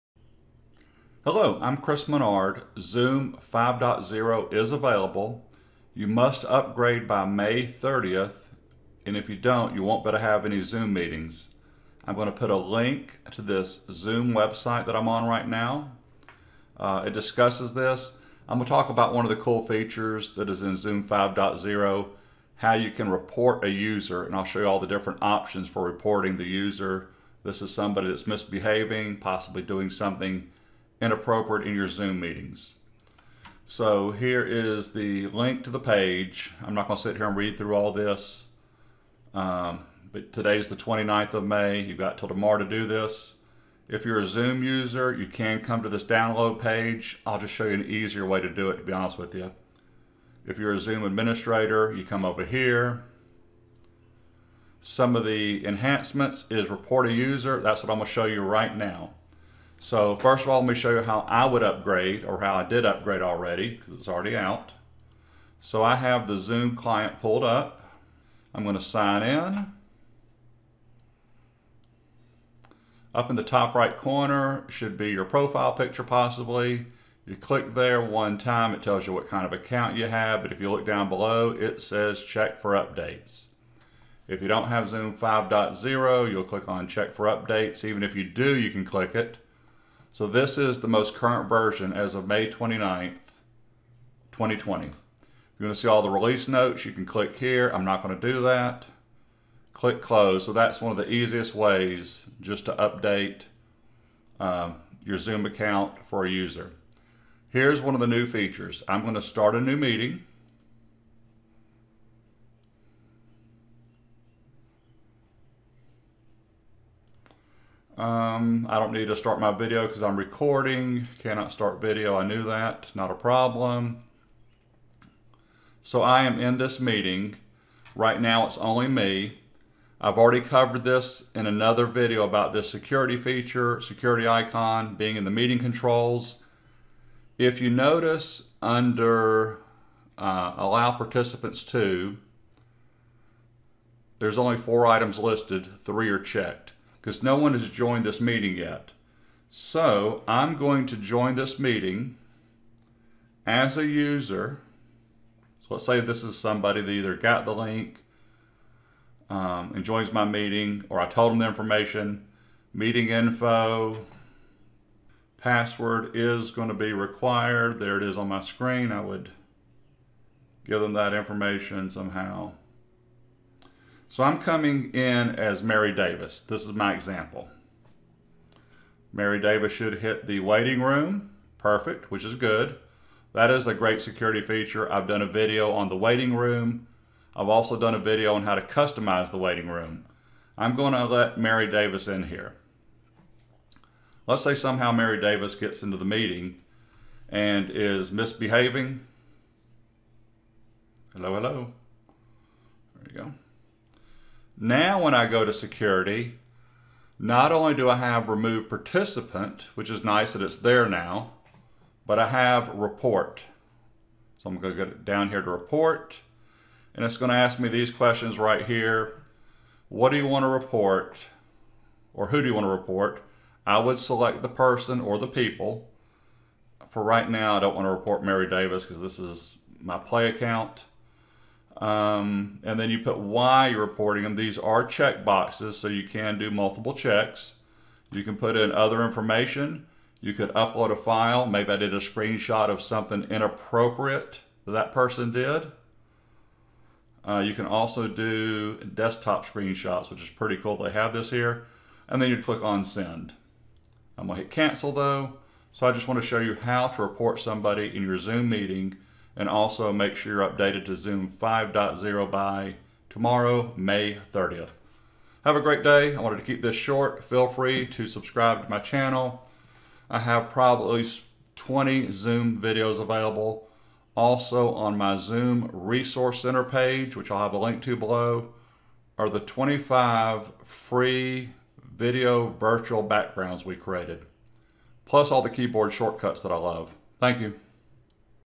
Updating to Zoom 5.0 & Reporting a user by Chris Menard
Starting May 30, 2020, Zoom users must be on client 5.0 in order to join a Zoom meeting. There are security enhancements in Zoom 5.0. Another new feature is the ability to report a user during a meeting. This is under the Security tab in the Meeting controls.

To report a user:
Click Security, click Report
Find the user to report and check the appropriate reasons for reporting.

-- EQUIPMENT USED
○ My camera